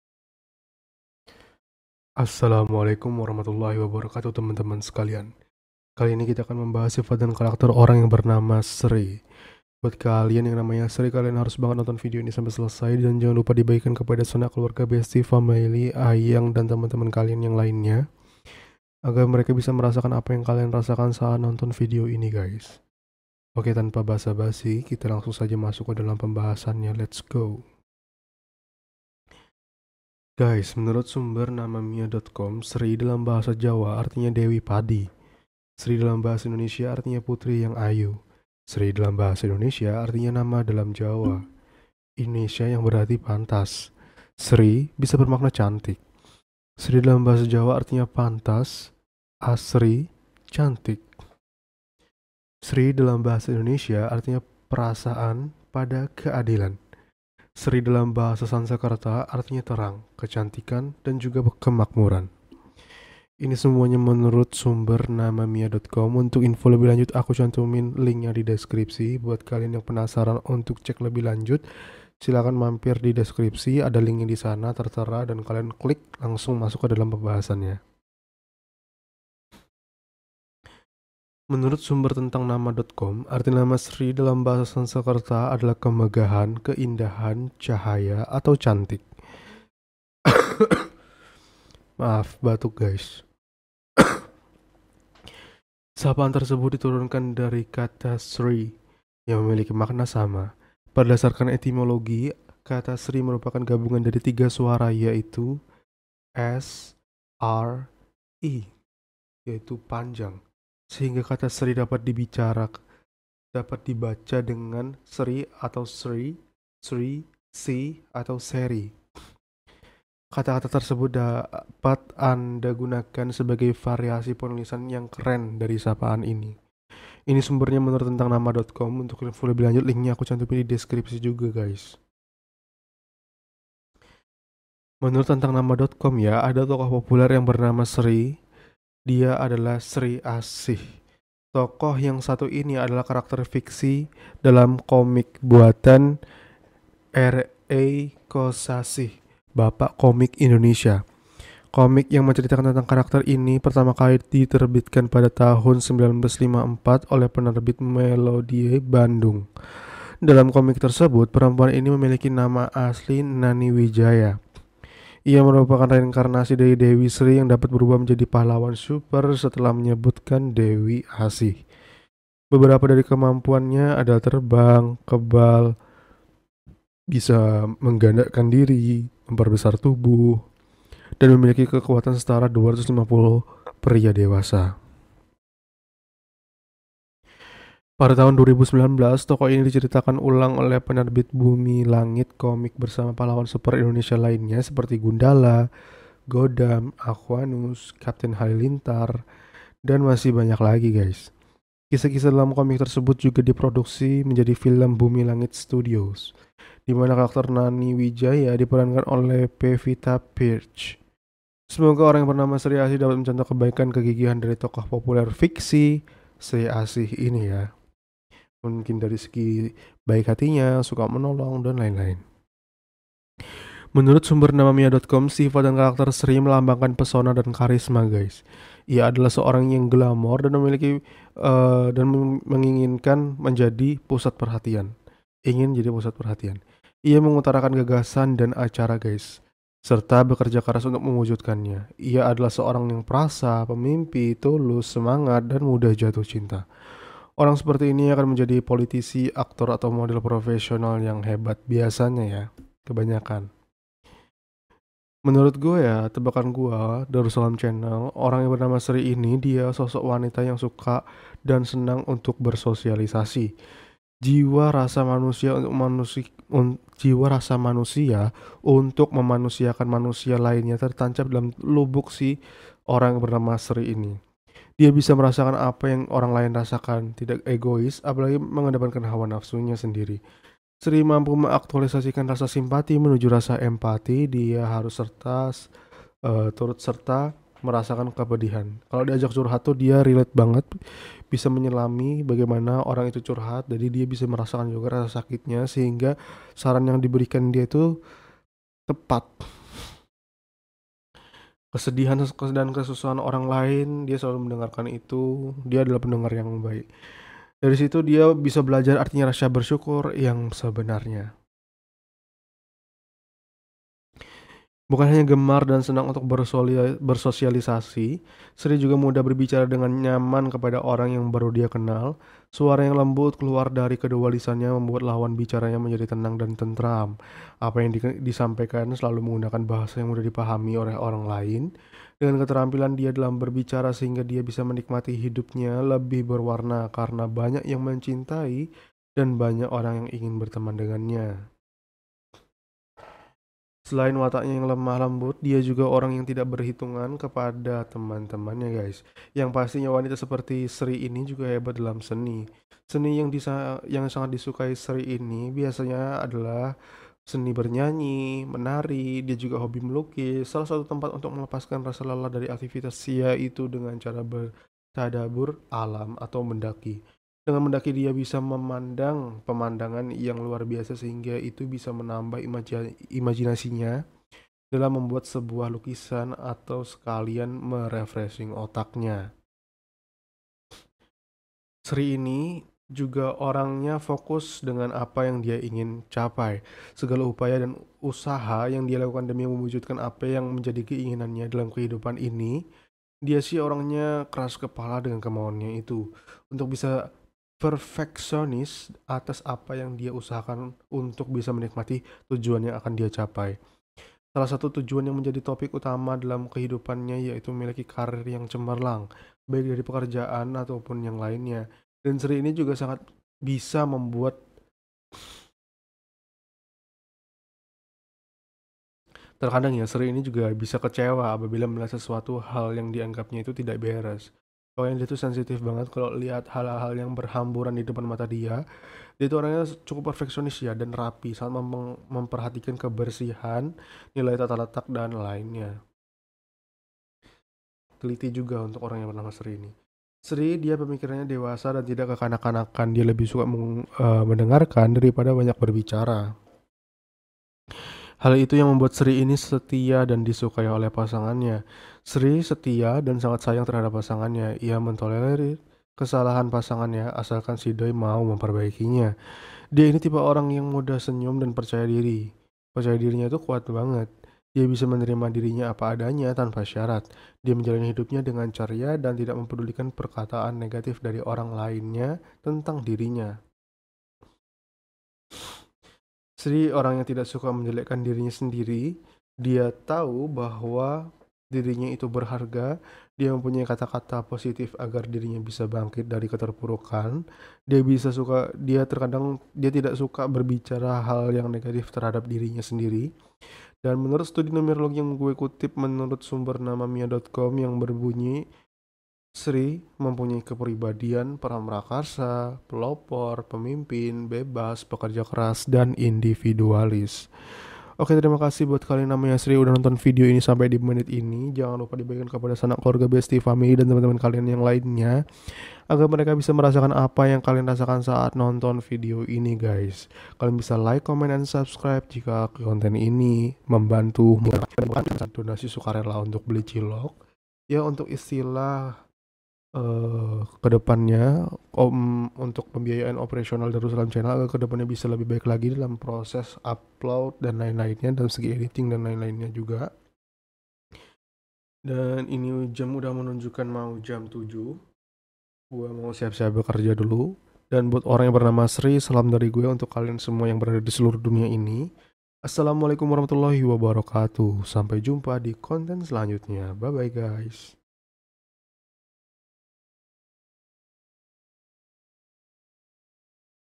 Nebak Sifat dan Karakter Nama Sri !
Follow Usaha Desain aku ya gaish
@darusssalamart ( S nya 3 ) makasih !

Menurut tebakan Darussalam Channel. Orang yang bernama Sri ini dia sosok wanita yang suka dan senang untuk bersosialisasi, jiwa rasa manusia untuk memanusiakan manusia lainnya tertancap dalam di lubuk hati seorang sri, dia bisa merasakan apa yang orang lain rasakan, tidak egois apa lagi mengedepankan hawa nafsunya sendiri. Sri mampu meaktualisasikan rasa simpati menuju rasa empati, dia harus turut serta merasakan kepedihan, kesedihan dan kesusahan orang lain, dari situ dia bisa belajar artinya rasa bersyukur yang sebenarnya.